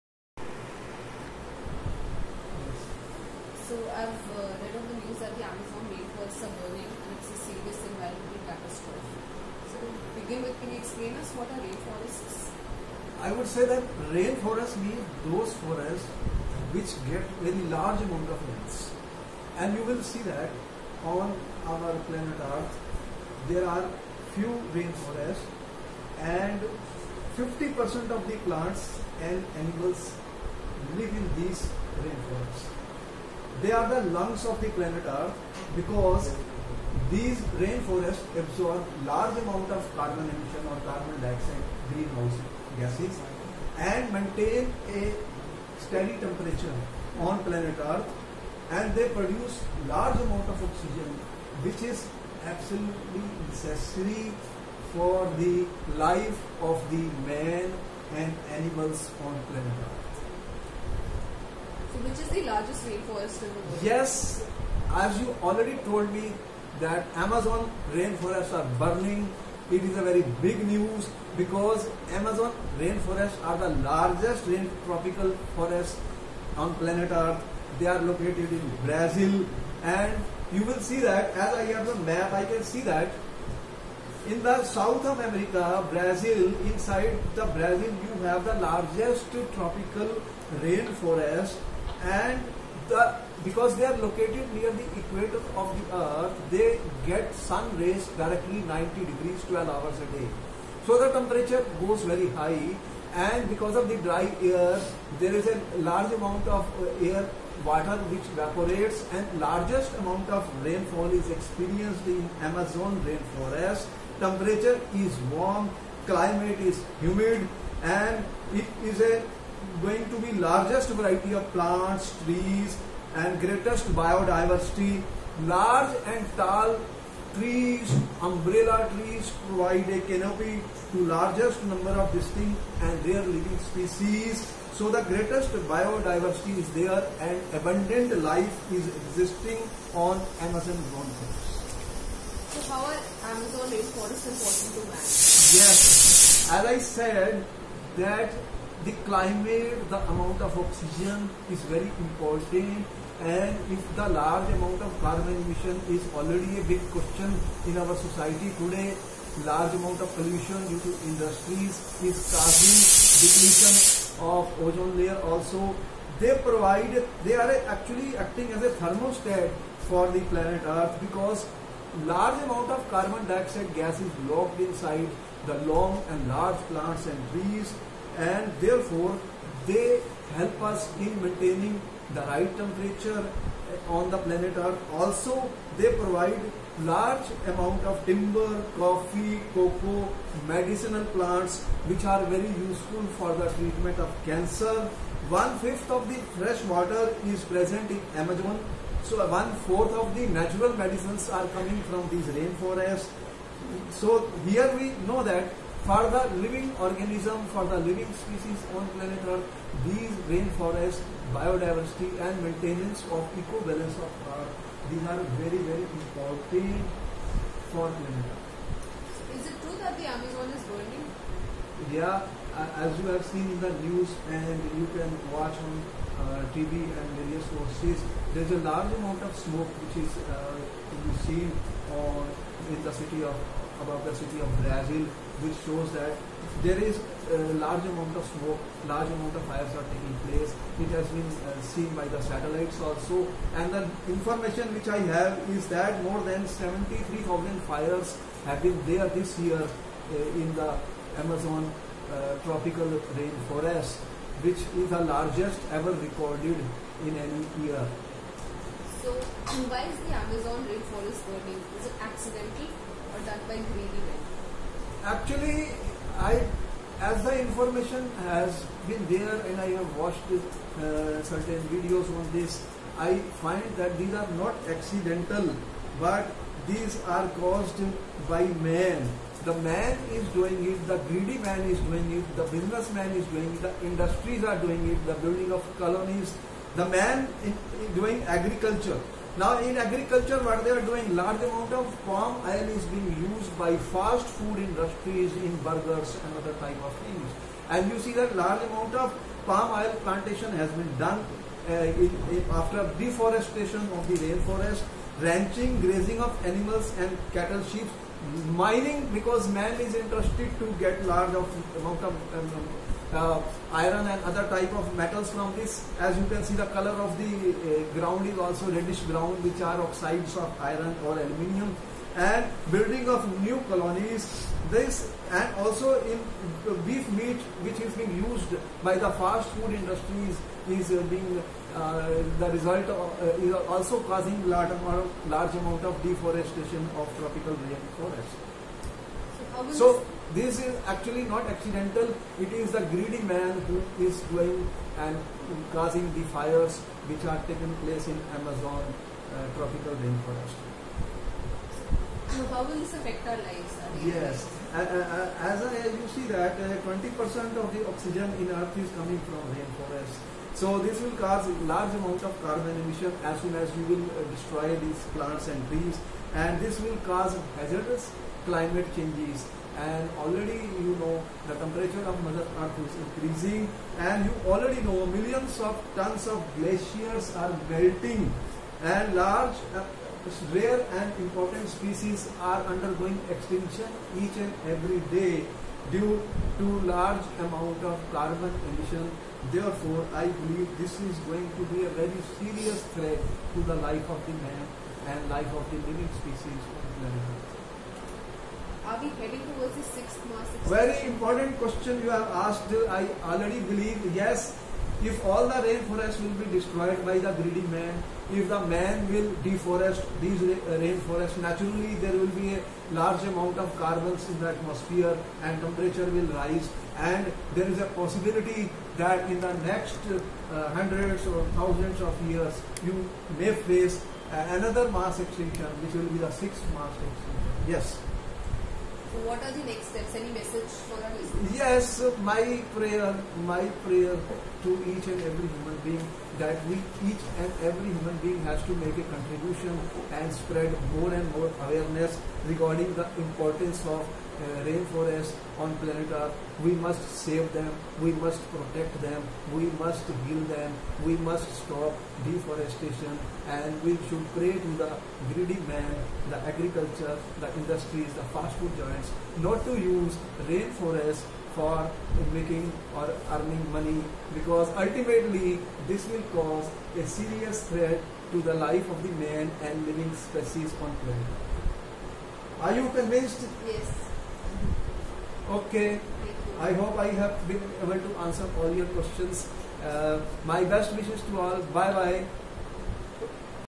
Amazon Fires Are An Environmental Emergency | Sunil Sikri
Amazon Tropical Rain-forests are burning. Rain forests are the lungs of earth. Rain forests lock large amount of carbon dioxide in trees and plants and provide about 20% oxygen to planet earth which is essential for the life of man and animals. Rain forests have abundant life and have greatest bio diversity. More than 72,000 fires have been reported this year. These fires are not accidental but caused due to Agriculture, Ranching, Mining, building new colonies by man for greed of man. Deforestation of rain  forests is releasing large amount of carbon in the atmosphere and is greatest environmental catastrophe. If rain forests will be destroyed by man due to greed of money, the life of man and living species is under serious threat and could lead to 6th mass extinction in future. This is health emergency and all out efforts be done to stop fires in the Amazon tropical rain forests.